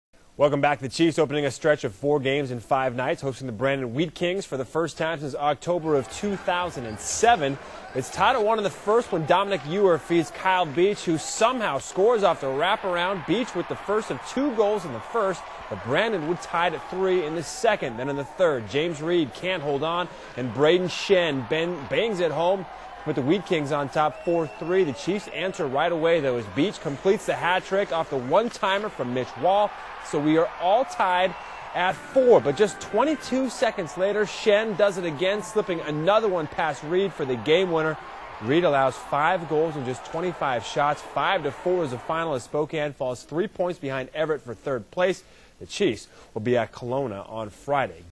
Chiefs lose offensive battle against Wheat Kings
Despite a big first period and a hat trick courtesy of Kyle Beach, the Spokane Chiefs dropped to the Brandon Wheat Kings by a score of 5-4 in the Spokane Arena on Wednesday. KXLY4's Derek Deis has more.